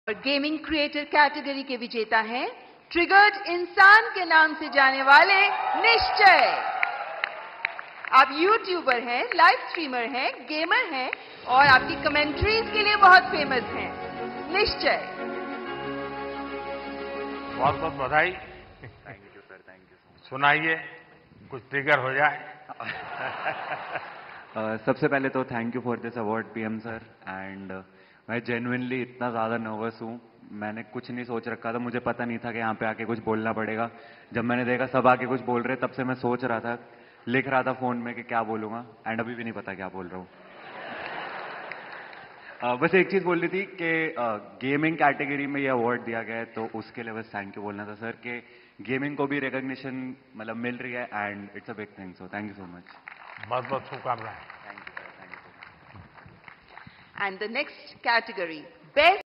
Triggered Insaan won the Creator award in best gaming category 🎉triggeredinsaan
Triggered Insaan won the award in best gaming category 🎉triggeredinsaan